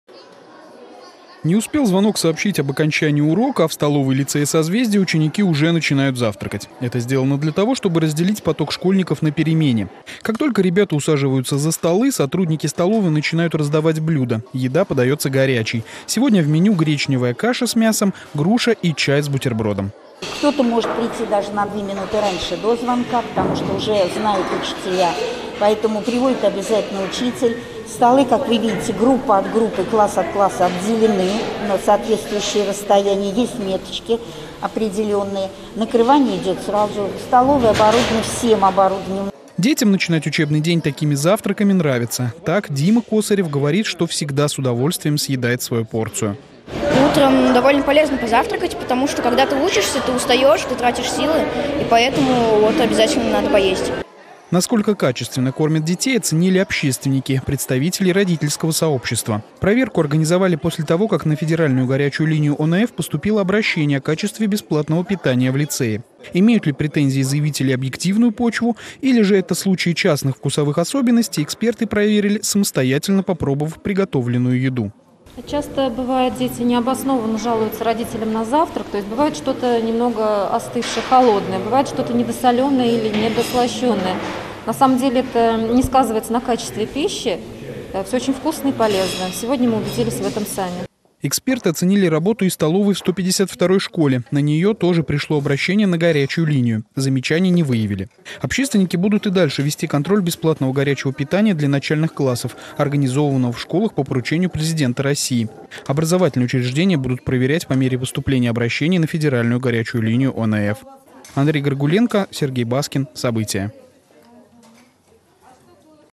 За качеством питания школьников регулярно следят общественники
Дело вкуса. В Самаре представители ОНФ и родители-общественники проверили качество бесплатного горячего питания в двух городских школах. На горячую линию Общероссийского народного фронта поступили обращения о спорном качестве блюд в этих учреждениях. Объективны ли эти претензии или это случаи вкусовых предпочтений, вместе с экспертами оценила и наша съемочная группа.